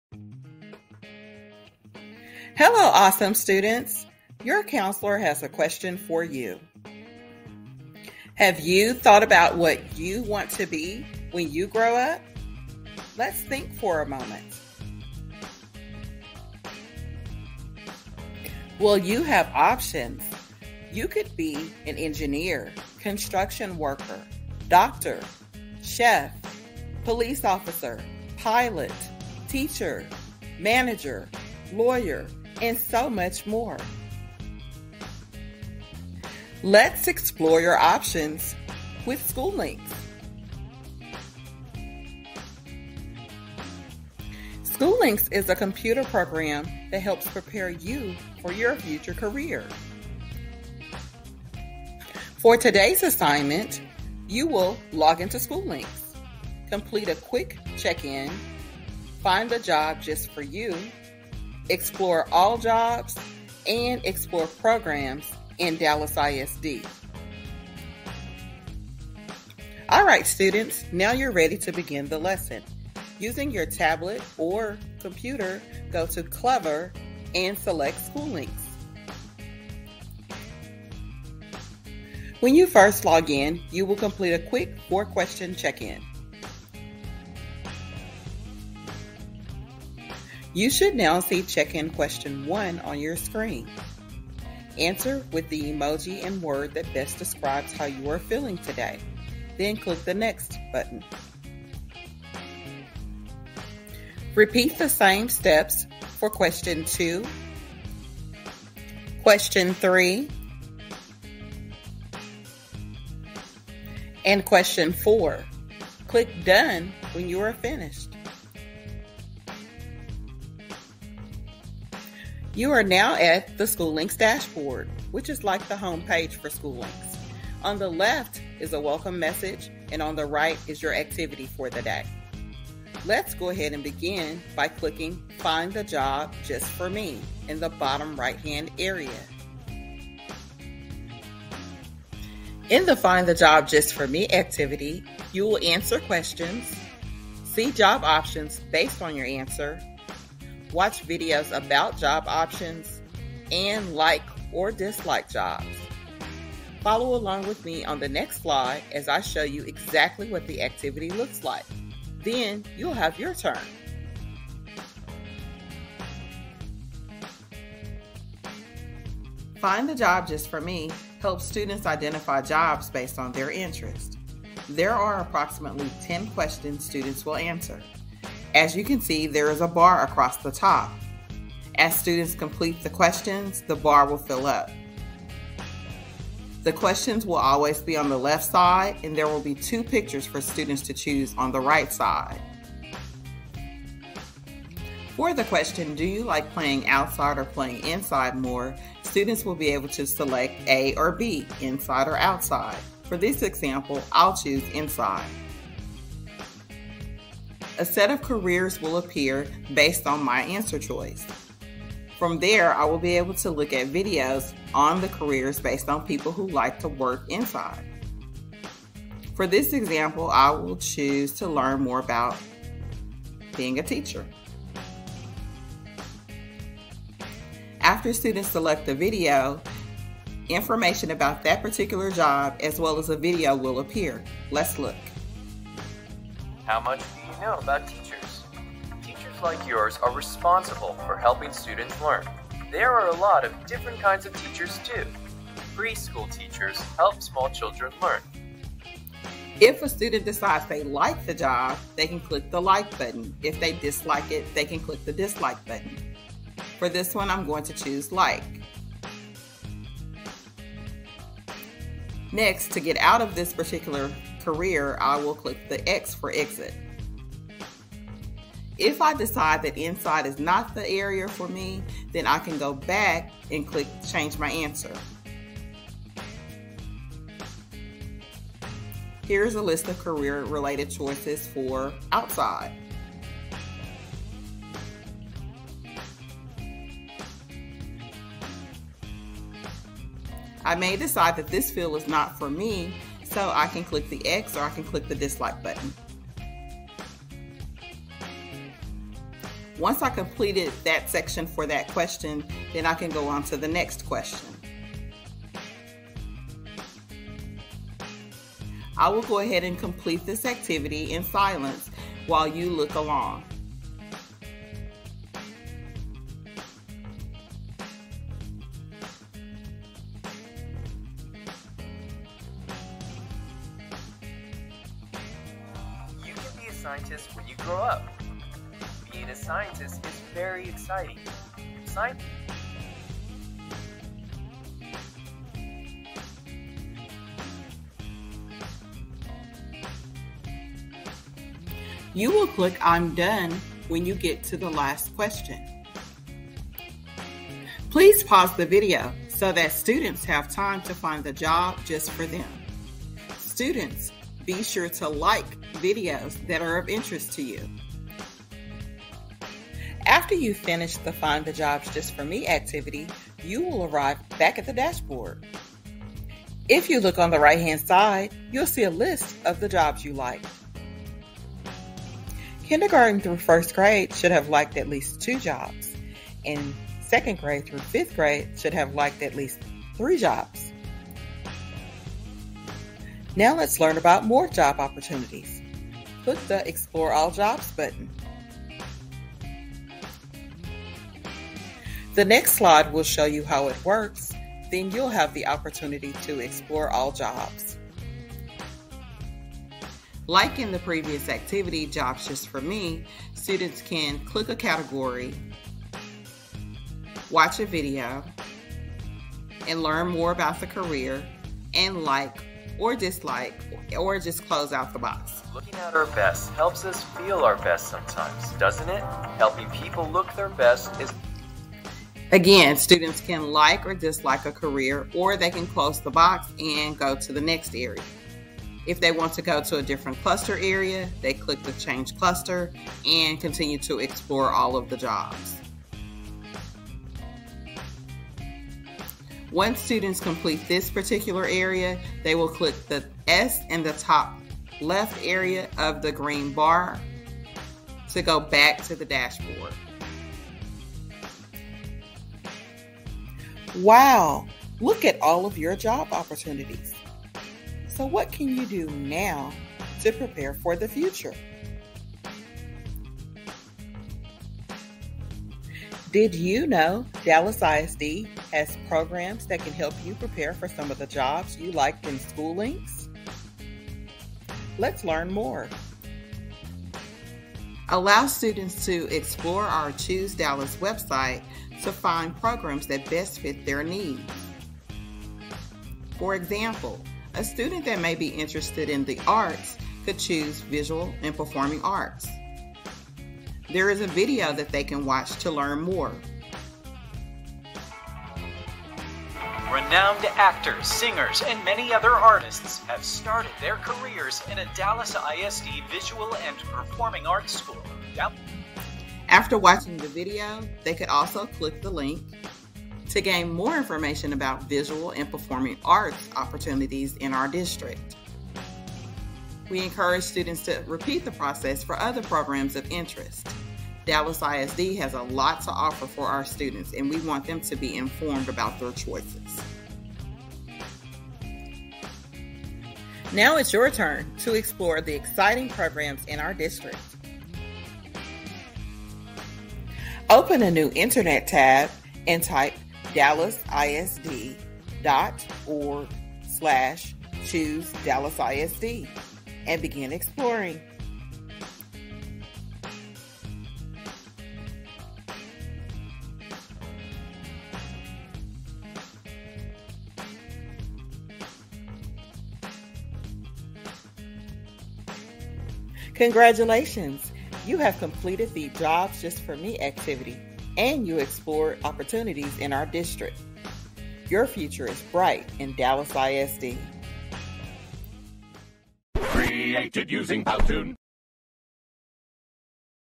SchooLinks for Students Video - Elementary
Help elementary students understand SchooLinks with this student-centered video.  Pause the video as needed to allow time for students to complete tasks.  Click Show More (below), to see time stamps for quick navigation.

Time Stamps:
0:00  Intro
0:18  Options
0:42  What is SchooLinks
0:51  Assignment
1:09  Login
1:22  Check-In
2:01  Dashboard
2:12  Jobs Just for Me Intro
2:47  Demonstration of  Find the Jobs Just for Me
5:47  PAUSE the video for independent work (set a time limit for students)
5:59  Jobs Liked and Explore All Jobs
7:45 Choose Dallas ISD
9:13  PAUSE the video for independent work (set a time limit for students)
9:25  Closing